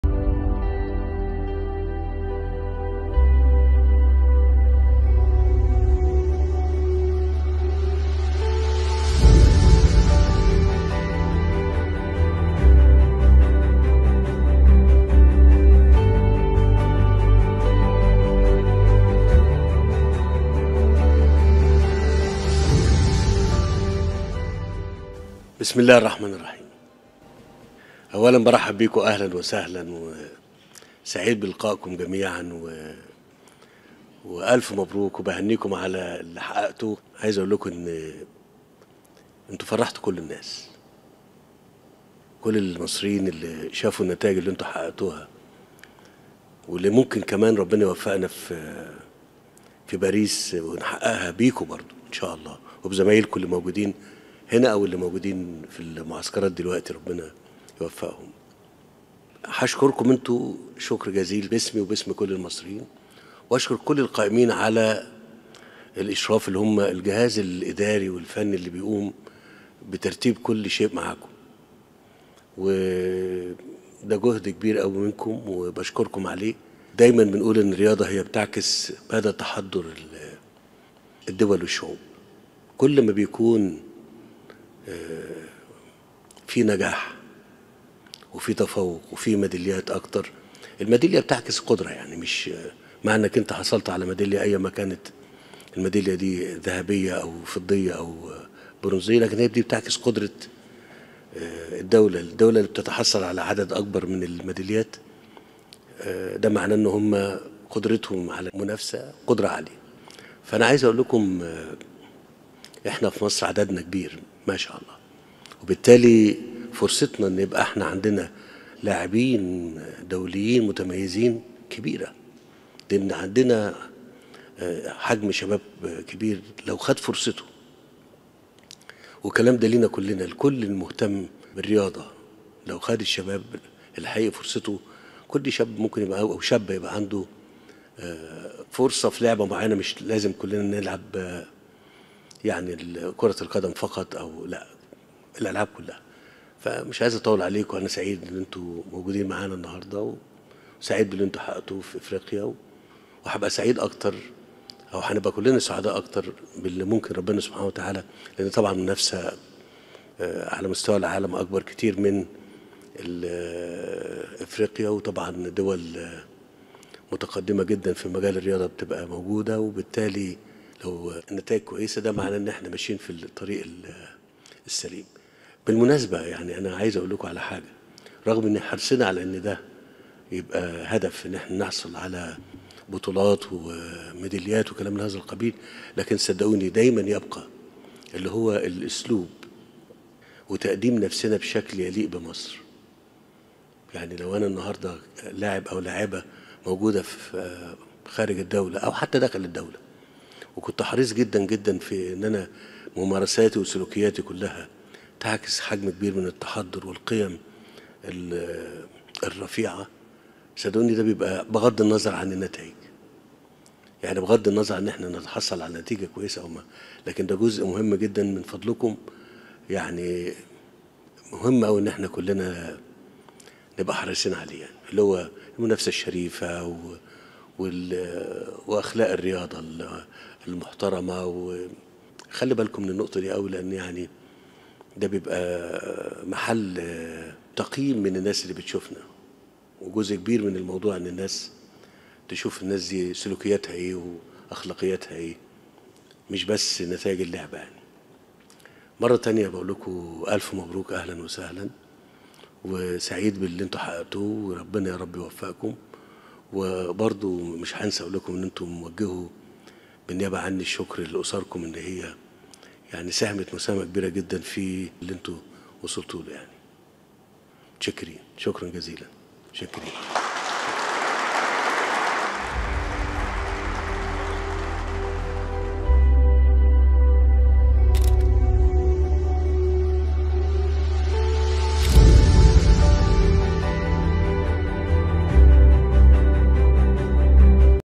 الرئيس السيسي يستقبل اللاعبين والمدربين الحاصلين على ميداليات ذهبية ببطولة الألعاب الأفريقية
الرئيس عبد الفتاح السيسي يستقبل اللاعبين والمدربين الحاصلين على ميداليات ذهبية ببطولة الألعاب الأفريقية تكريماً لهم